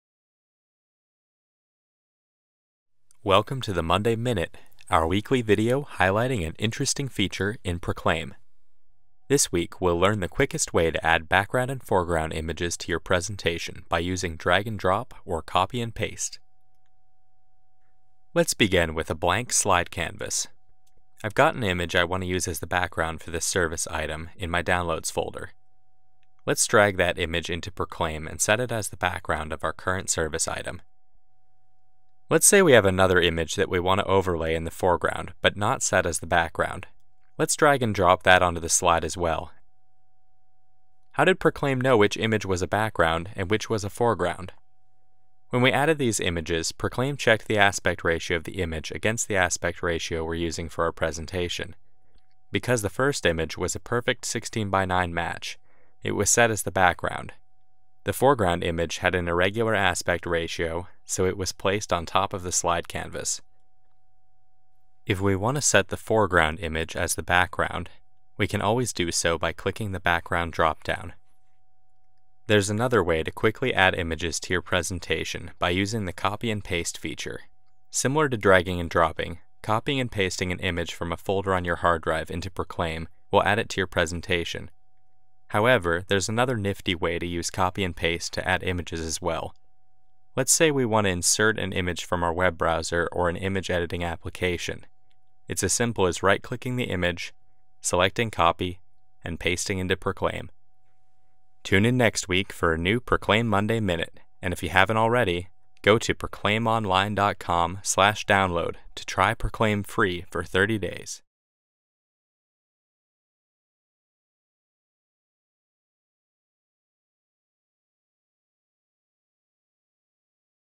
Drag & Drop and Copy & Paste - Proclaim Monday Minute
In this week's Monday Minute, you'll learn how to Drag & Drop or Copy & Paste images right into Proclaim!
Haven't tried Proclaim yet?